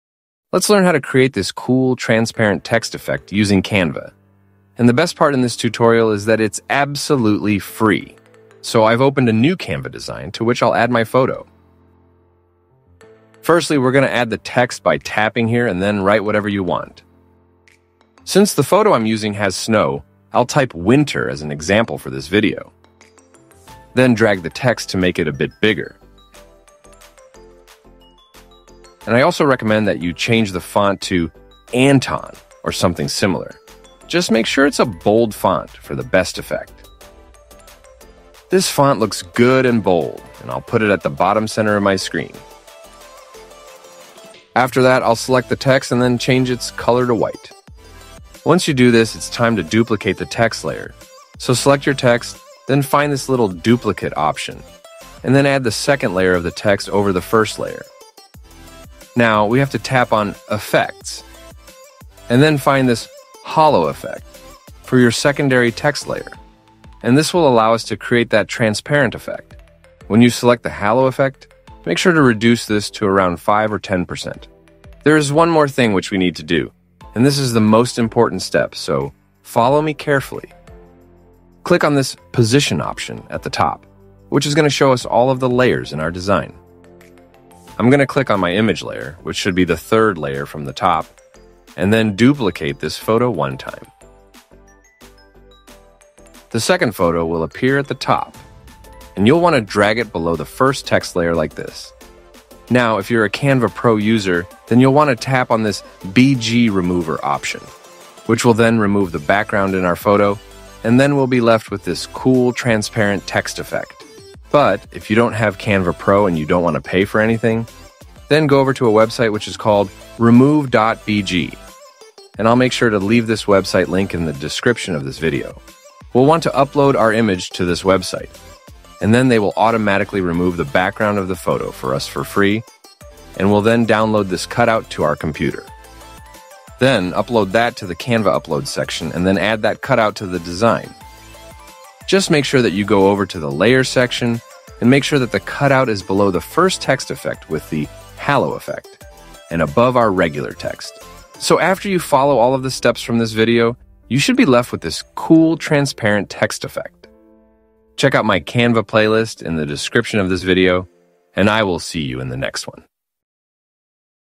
How to Create Transparent Text Effect in Canva
How to Create a Transparent Text Effect in Canva (FREE & Easy!)

Want to create a cool transparent text effect in Canva? In this tutorial, I’ll show you how to achieve this effect step by step—for FREE, no Canva Pro needed!

🔹 What You’ll Learn in This Video:
✔️ How to add and style text in Canva
✔️ How to create a transparent (hollow) text effect
✔️ How to remove the background (free & Canva Pro methods)
✔️ Best font choices for a bold, professional look

This music is free to use under the terms provided by the original creator, but I do not claim any ownership over it.